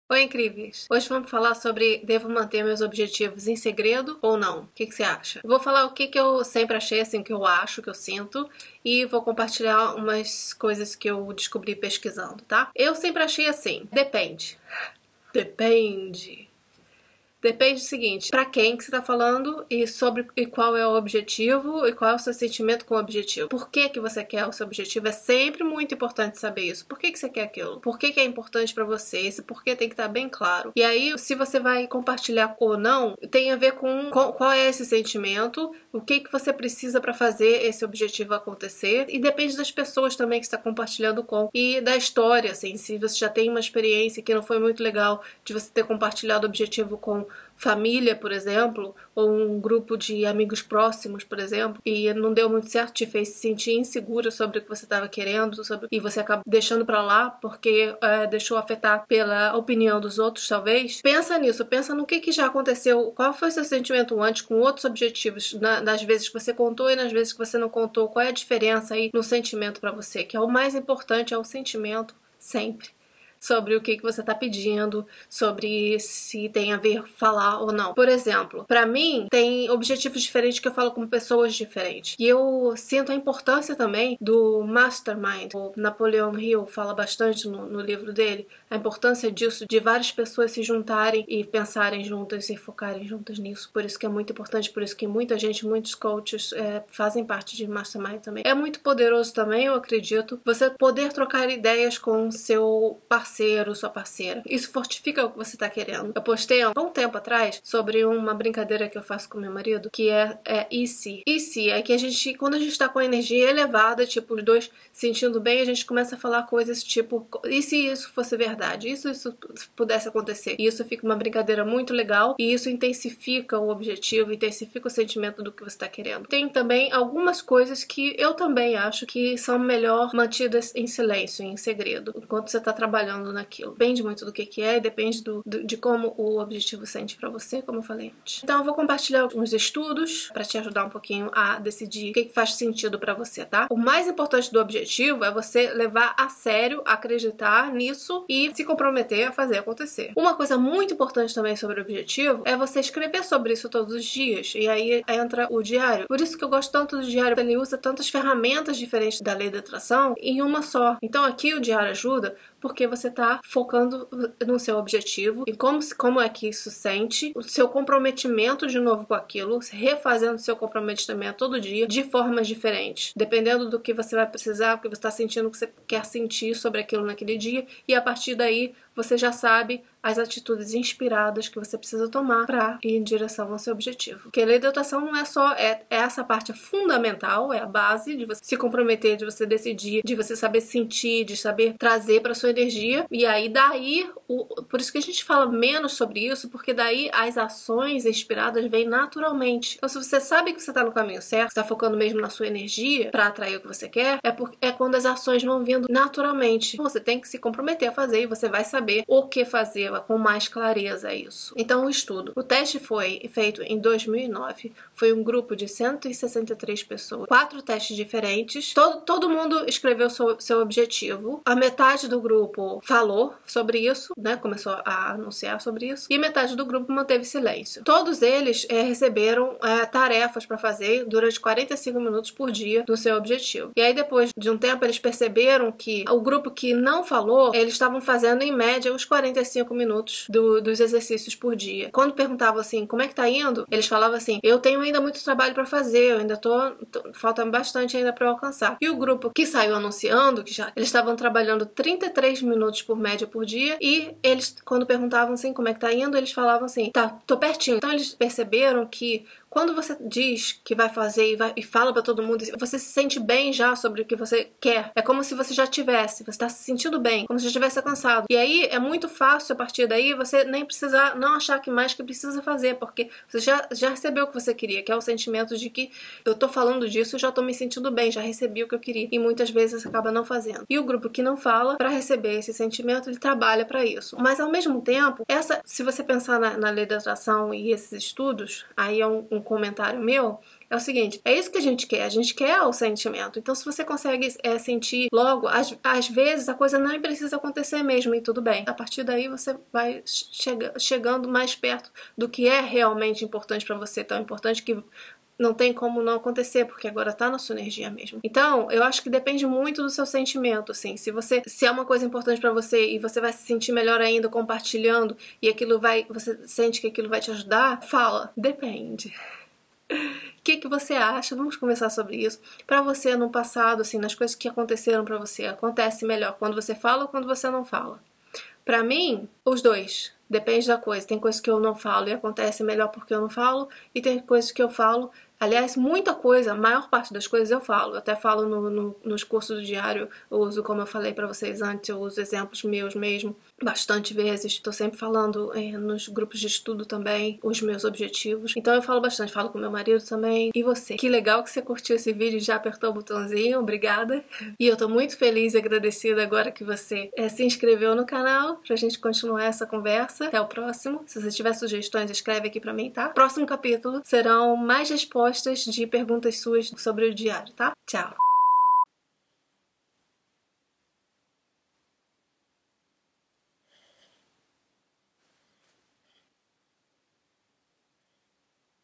Manter segredo sobre seu objetivo?
Vamos conversar sobre o que funciona melhor, manter segredo ou contar sobre seus objetivos?